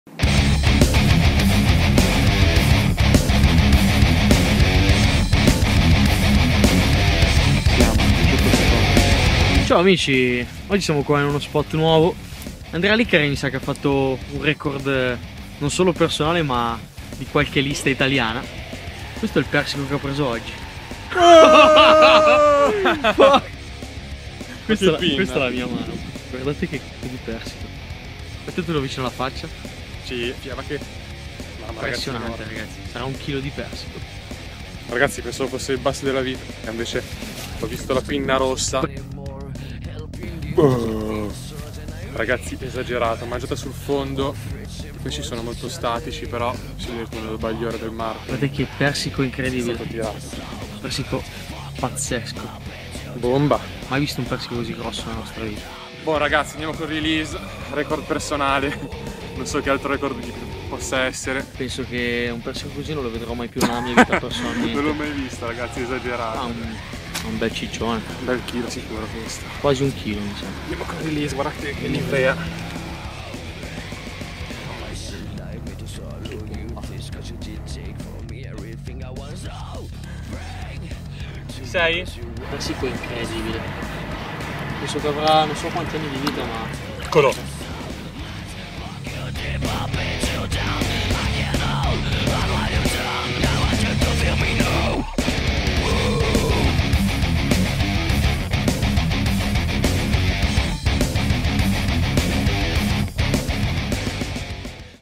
BIG PERCH, persico reale record
Persico reale di taglia pescato in acque libere. 43cm x 1kg